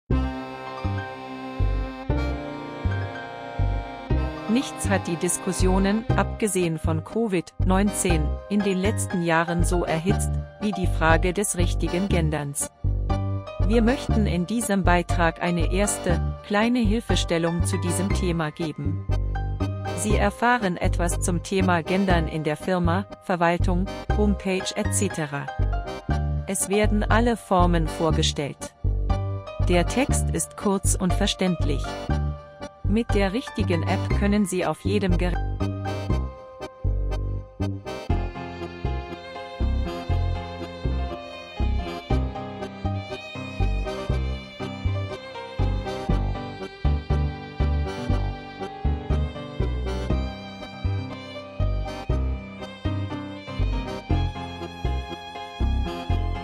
GfA-Info to Go - Die Fallstricker der Diversität
dies ist ein kleiner Hinweis zu unserem E-Book "Die Fallstricke der Diversität: Gendern - aber wie?"